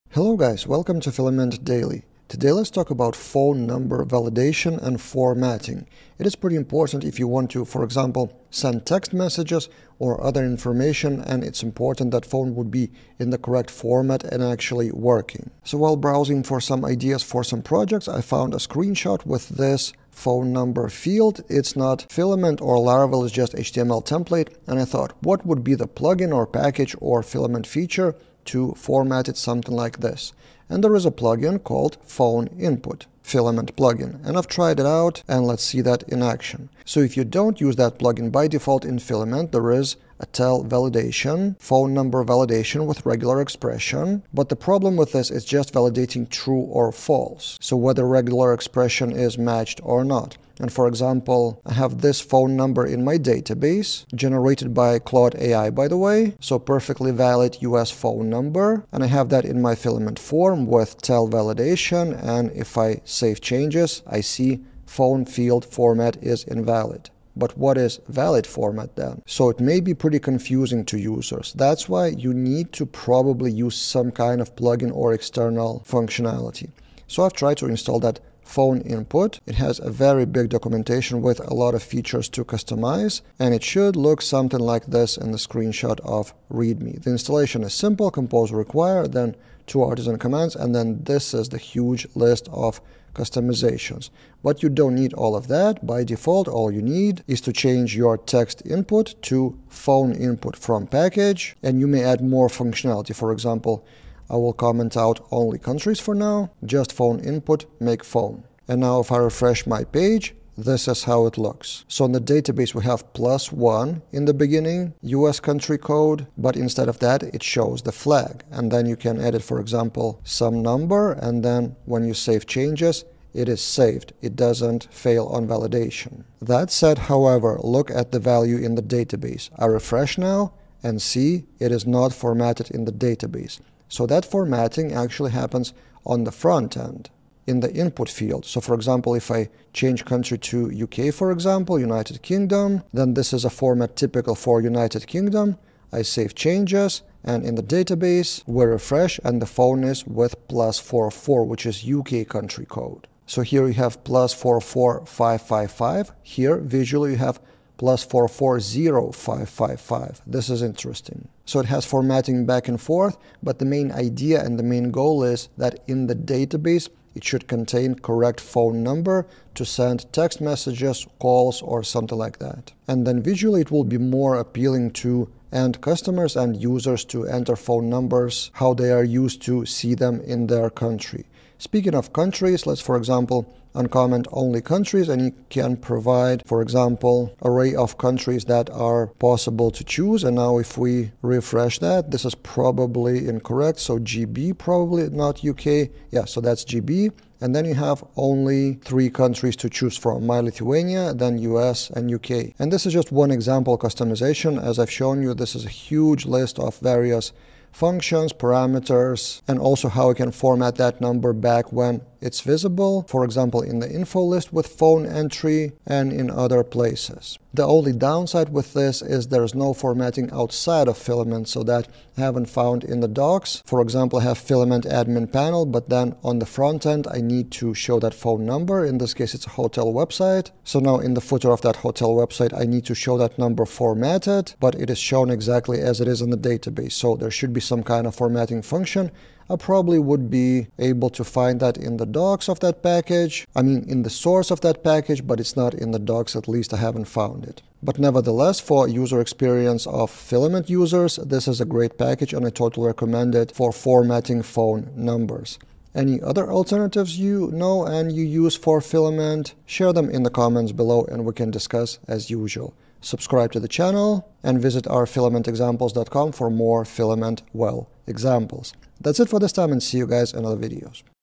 Filament Phone Input Plugin: Quick Demo
Demonstration of the default Filament phone validation versus using a specific plugin.